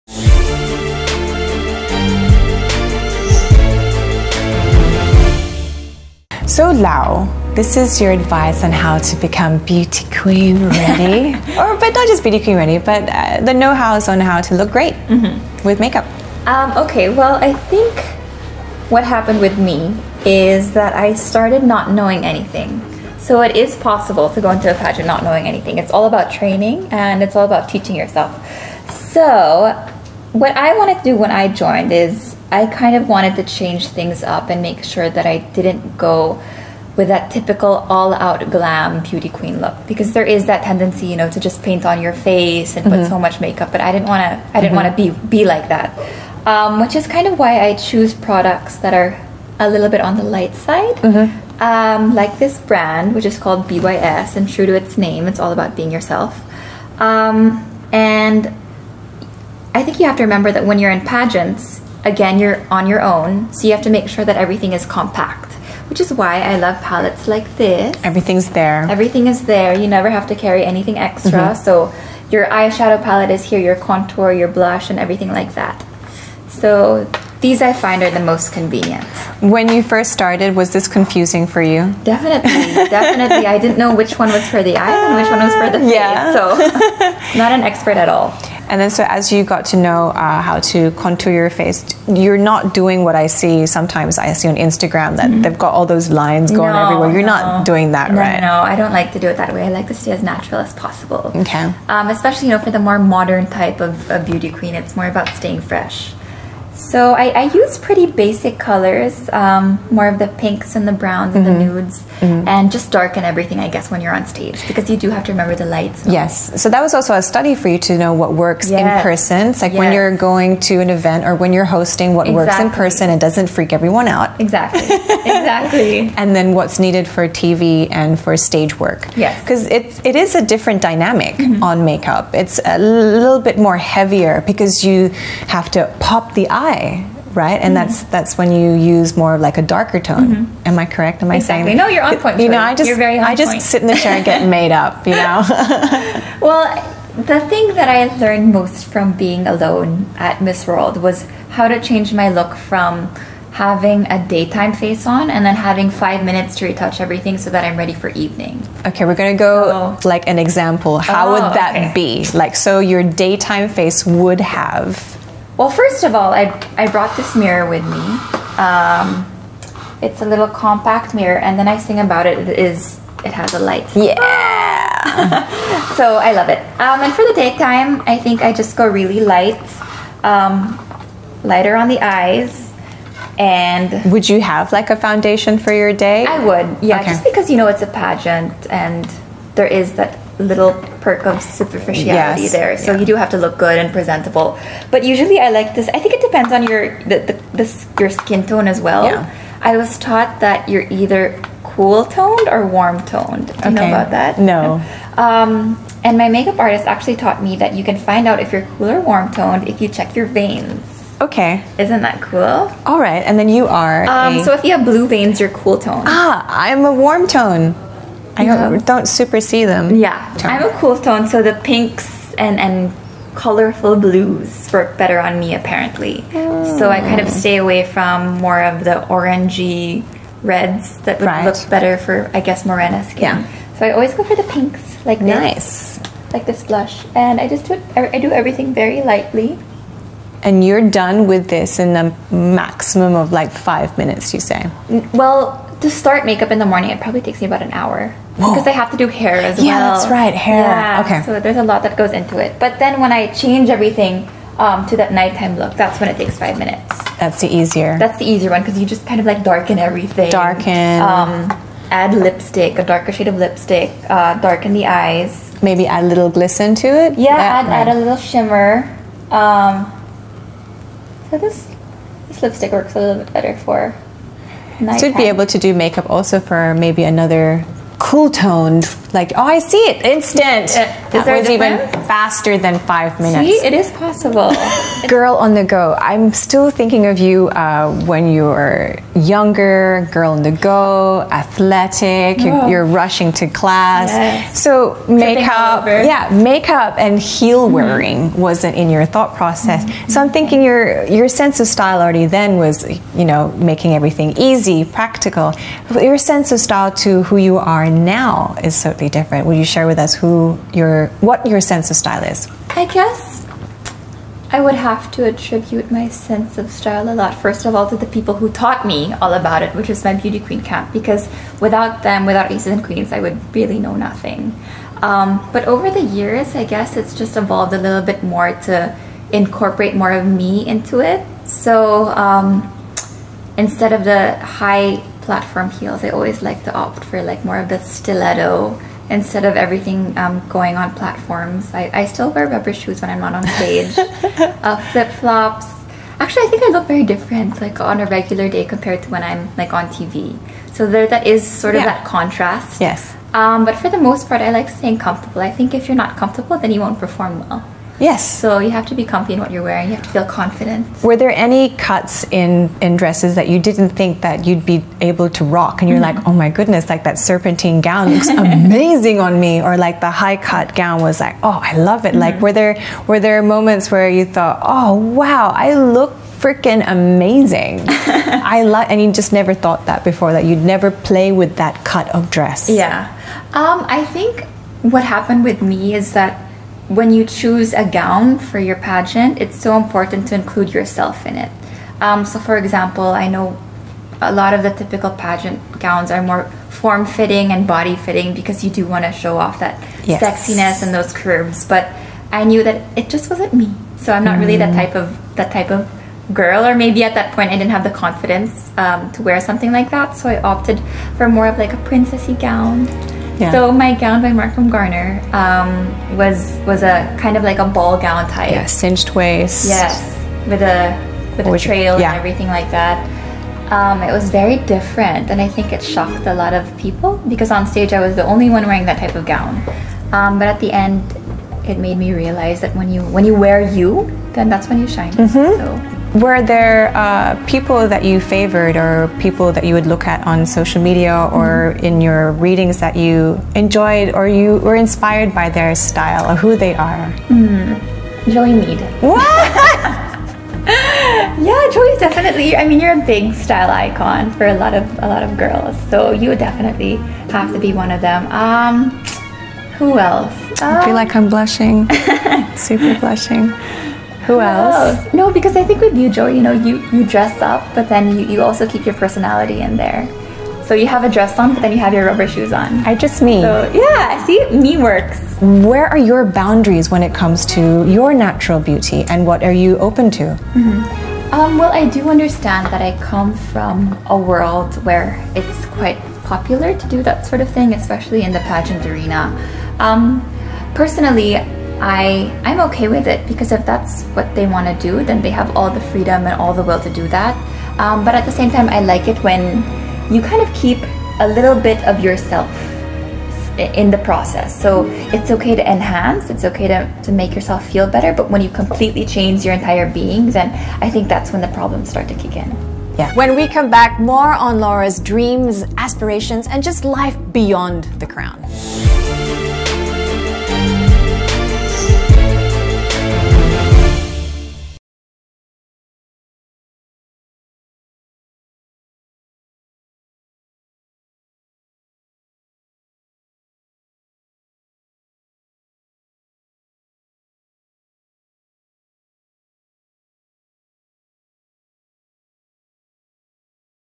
Laura Lehmann knows how and how to look great in make up | Women of Style
How to look great in make up with Laura Lehmann.